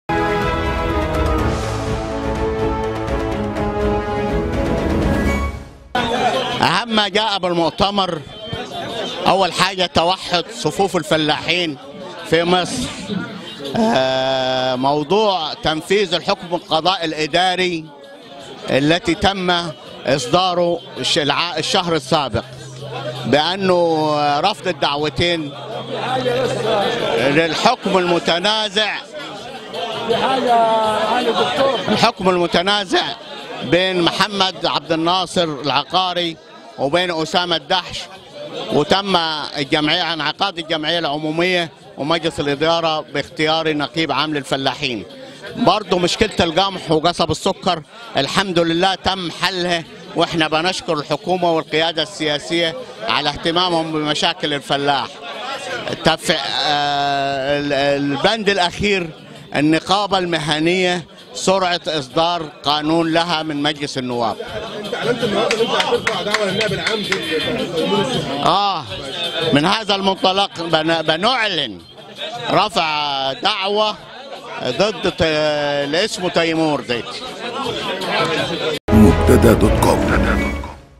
نقيب الفلاحين يهُدد السبكى.. ويؤكد: «صفوفنا موحدة»
قال محمد راشد أبو الوفا، القائم بأعمال النقيب العام للفلاحين، توحد صفوف الفلاحين بمصر، وذلك عقب تنفيذ الحكم القضاء الإدارى الذى تم اصداره الشهر السابق برفض الدعوتين للحكم المتنازع عليه بين محمد عبد الناصر العقارى وأسامة الجحش وتم انعقاد الجمعية واختيار نقيب عام للفلاحين.

وأشار أبو الوفا، إلى مشكلة القمح وقصب السكر، قائلًا نشكر القيادة السياسية والحكومة على اهتمامهم بمشاكل الفلاح، مطالبًا بسرعة إصدار قانون للنقابة المهنية للفلاحين من مجلس النواب.

وأكد نقيب الفلاحين على رفع دعوى ضد تيمور السبكى، لما بدر منه من إساءة لنساء الصعيد.